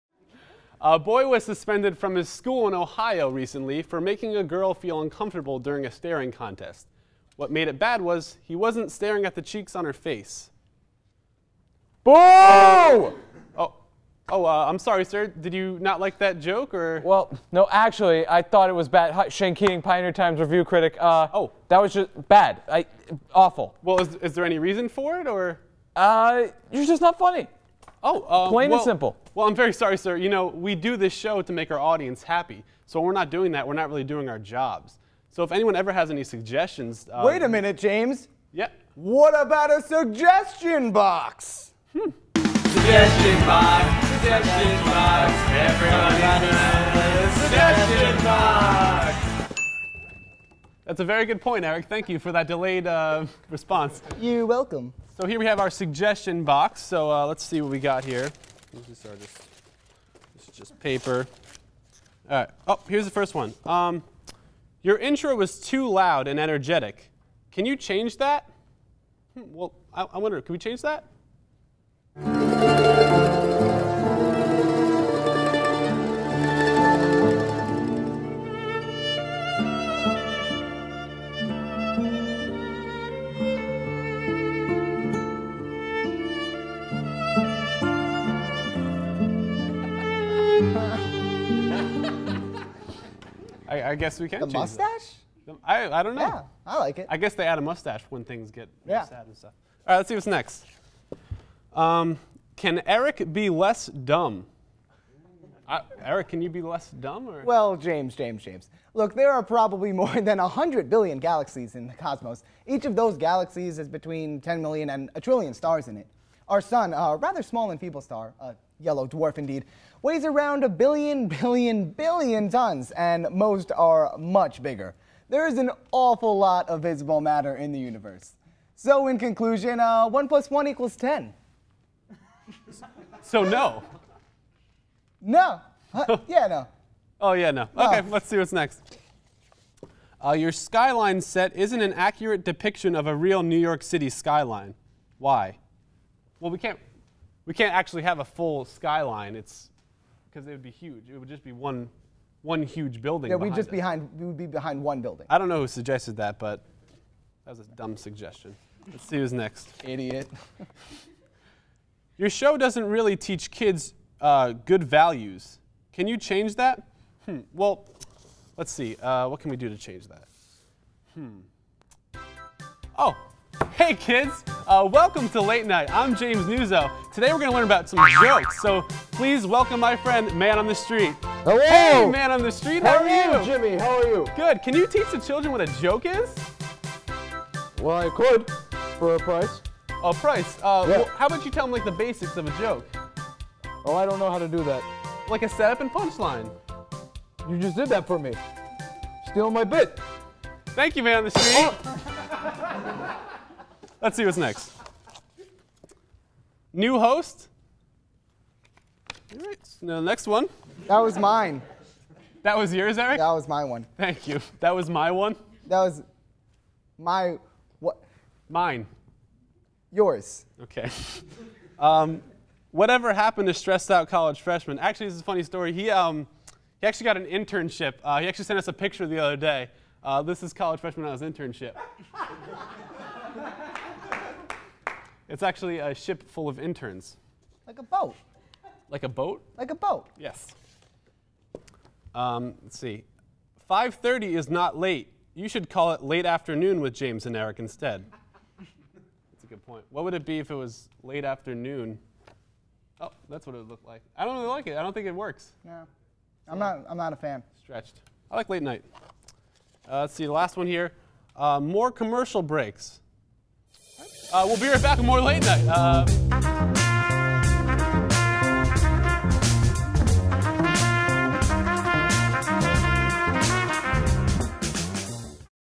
Suggestion Box
In order to please one of our critics, James and Eric start taking some suggestions from our Suggestion Box!